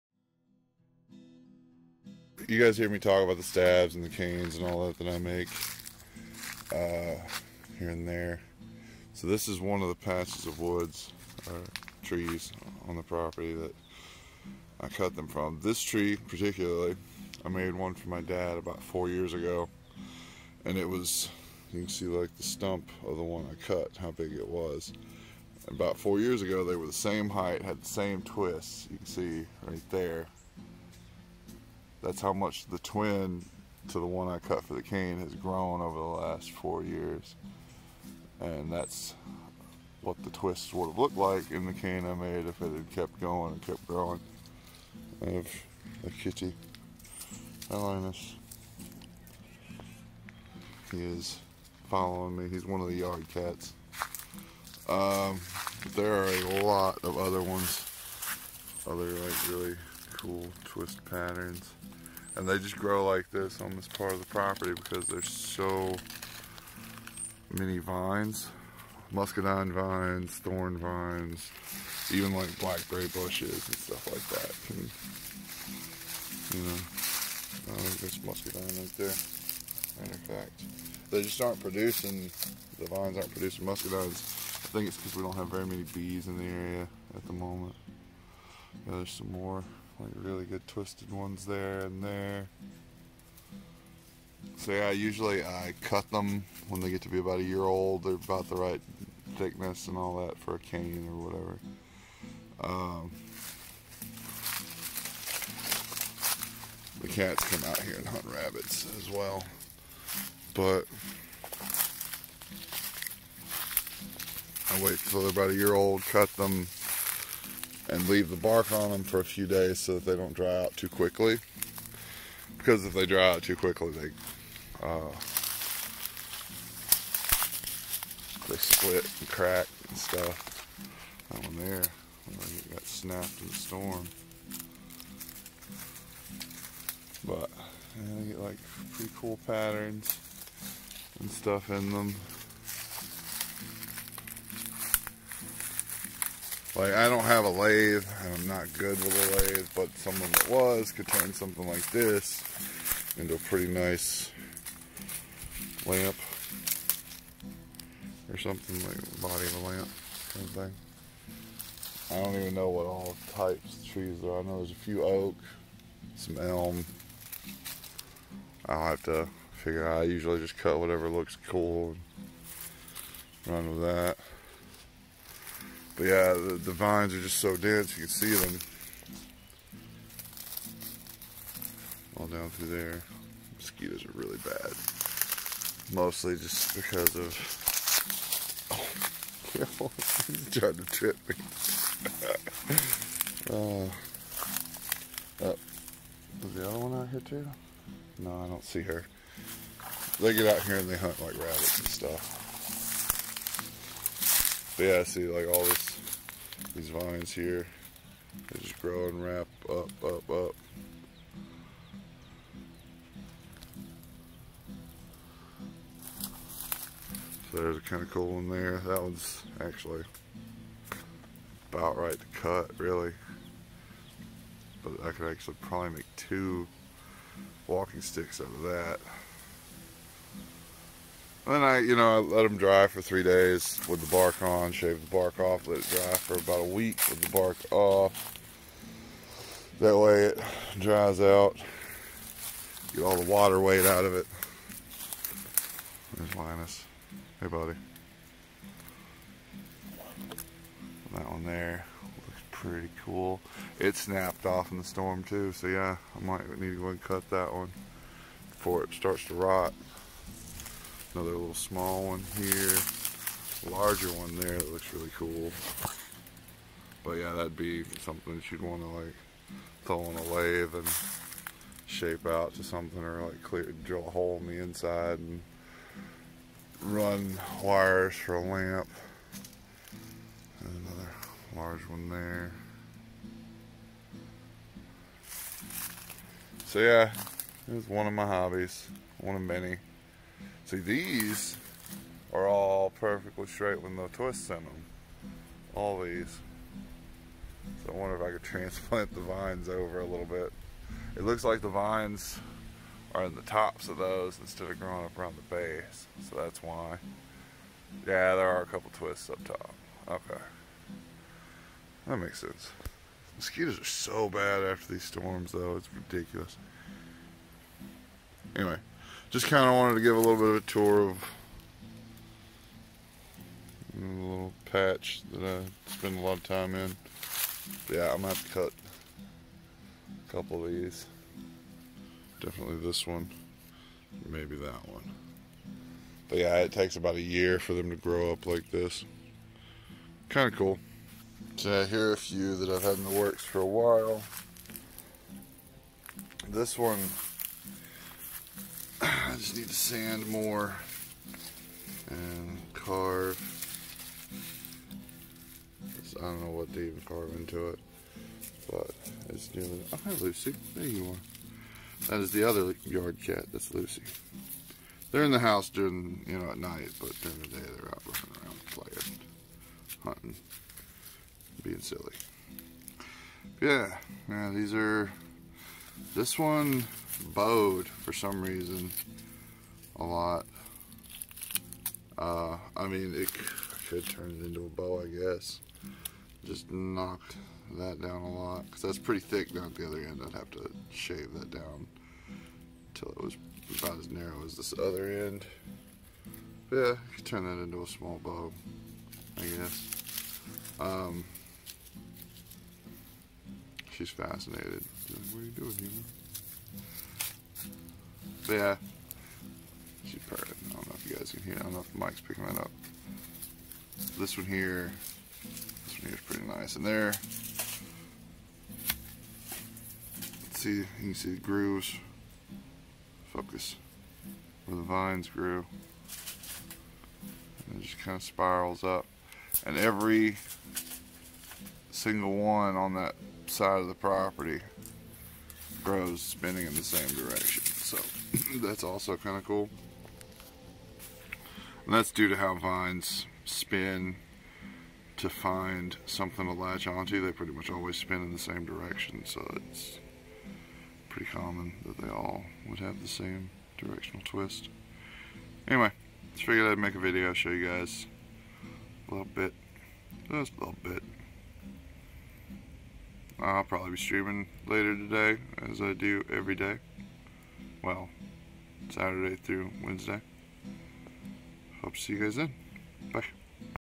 Walking Sticks.
A lil more about the canes/walking sticks that I make. I live in an area that's pretty overrun by snakes so they can also be very useful for dealing with them.